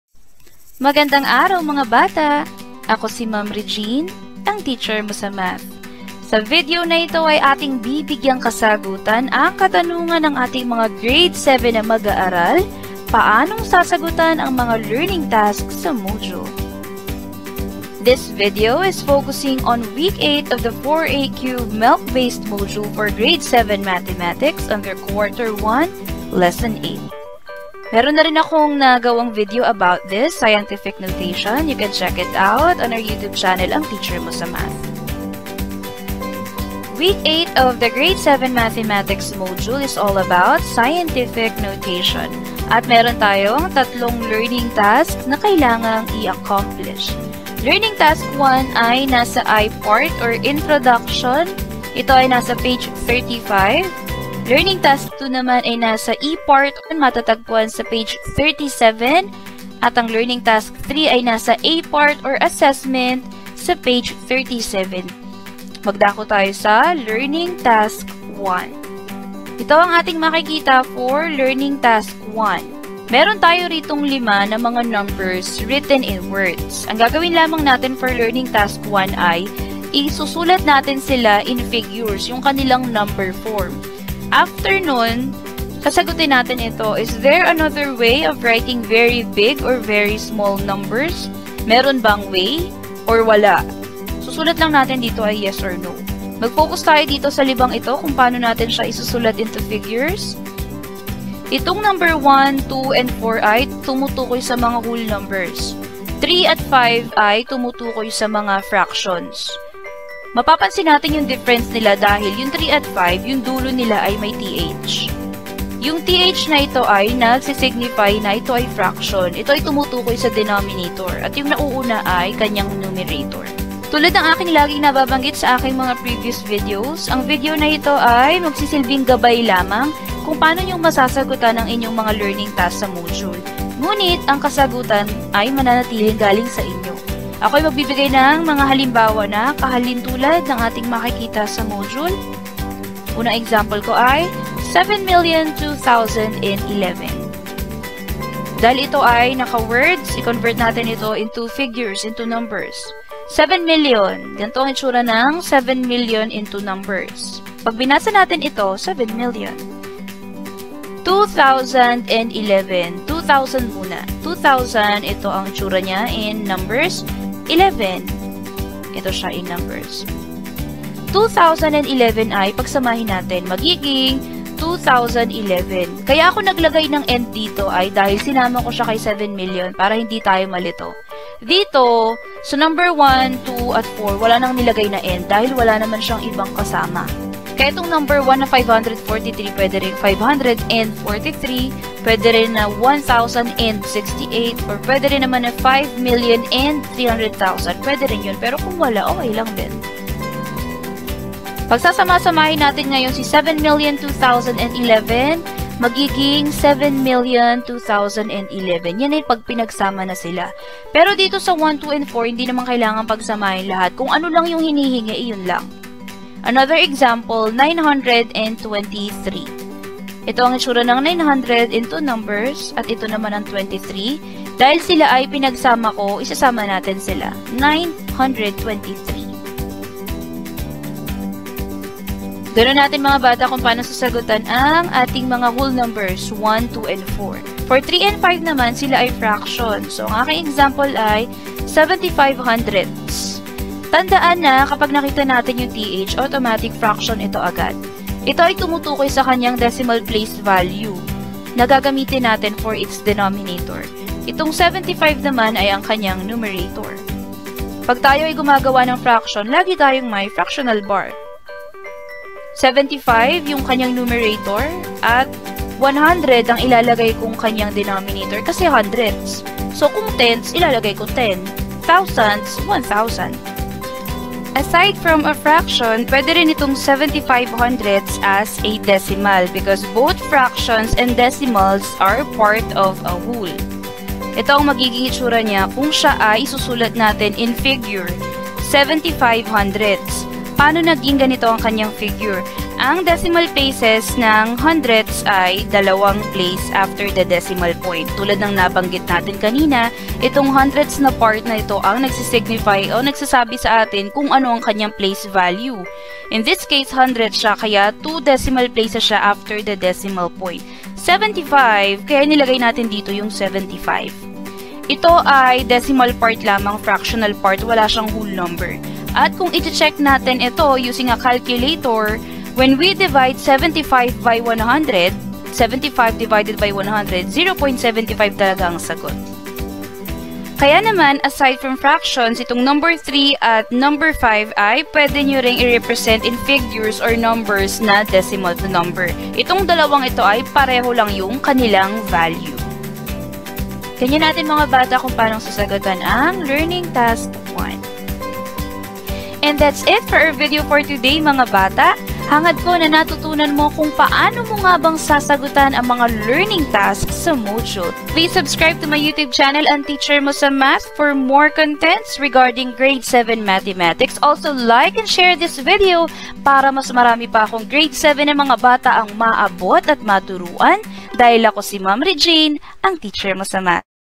Paano sagutan ang module? Q1 WEEK 8 Learning Task 1 (G7 Mathematics) TAGLISH
This video intends to asynchronously discuss how to answer Learning Task 1 of Quarter 1 Week 8 from the MELCs-based (4A QuBe) module for G7 Mathematics under Quarter 1 Lesson 8 about Scientific Notation.

Q1 Week 8 Learning Task 1 page 35

Links:
PAANO SAGUTAN ANG MODULE FOR WEEK 8?

(NEXT LESSON !!!) Q2 WEEK 1 LESSON: